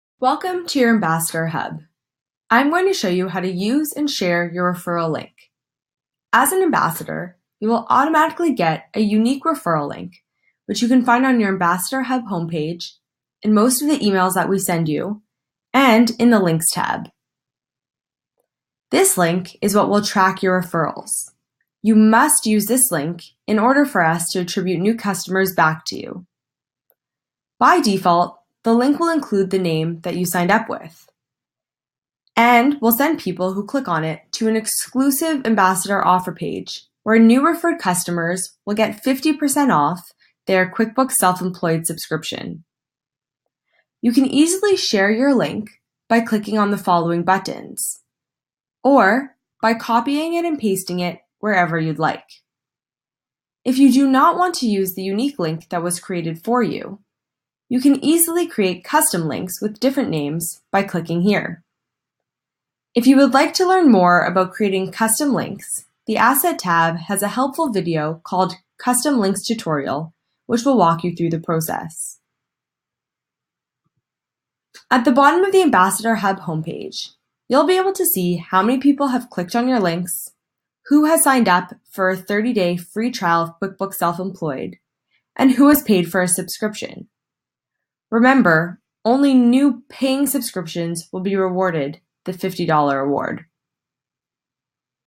How To QuickBooks Self Employed Ambassador Hub
Get comfortable navigating your Ambassador Hub and learn how to use and share your unique ambassador link.